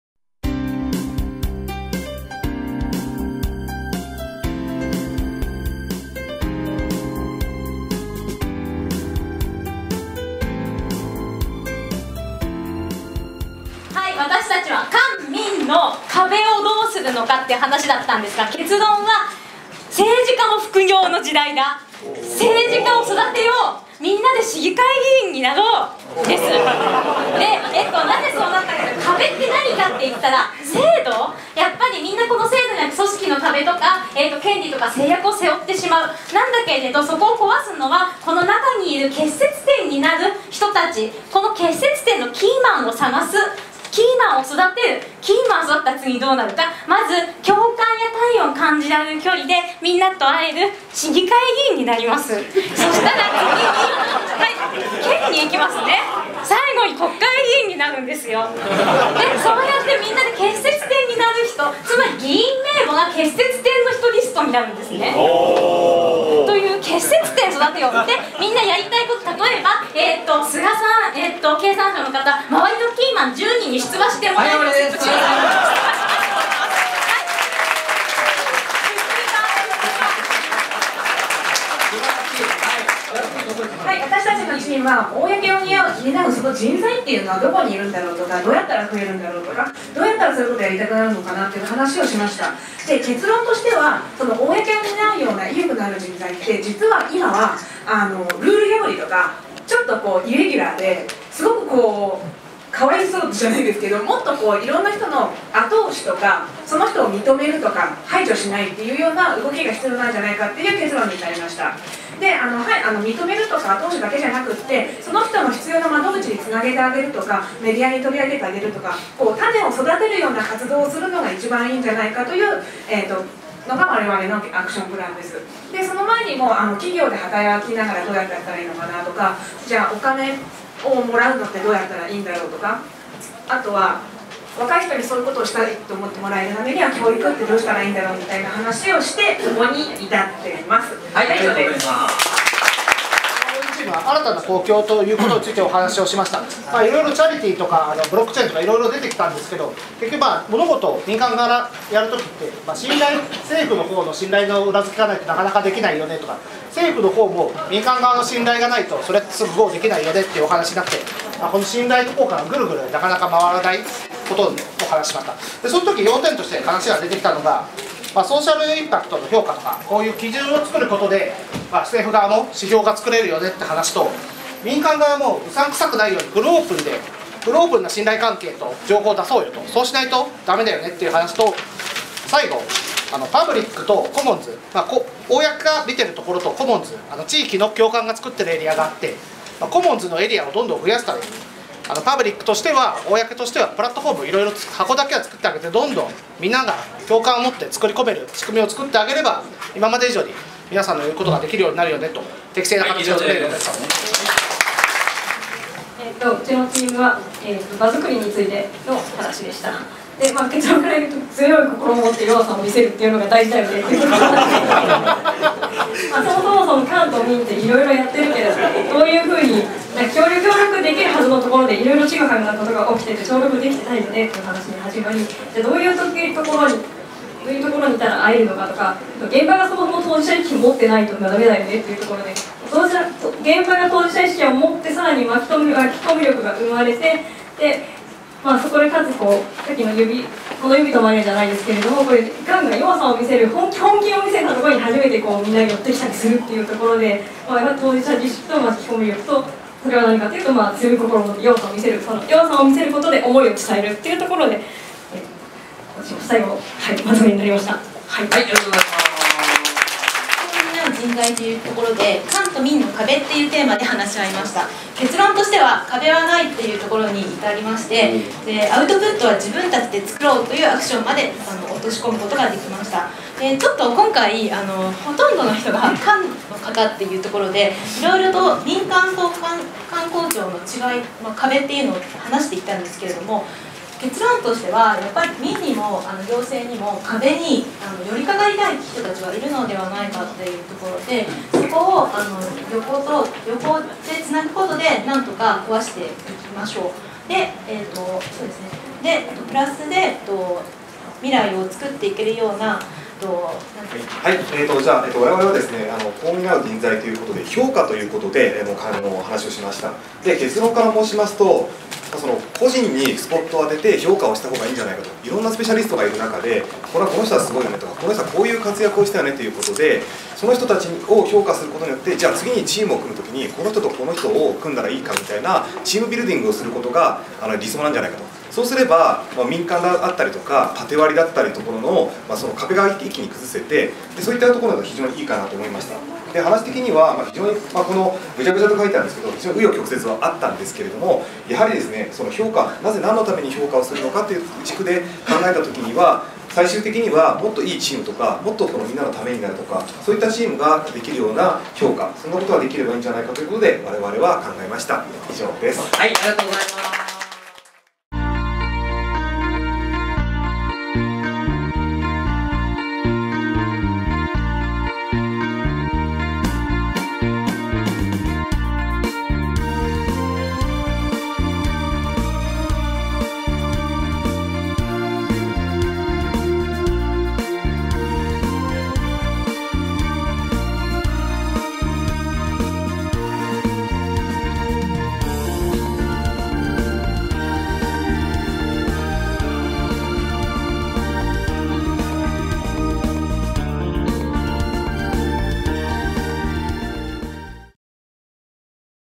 立ちすくむ国家WS_20170613-4 結果発表_公を担う人材
2017/6/13に開催された「立ちすくむ国家ワークショップ」で行われた3つのテーマのうち「公を担う人材」に関する結果発表です。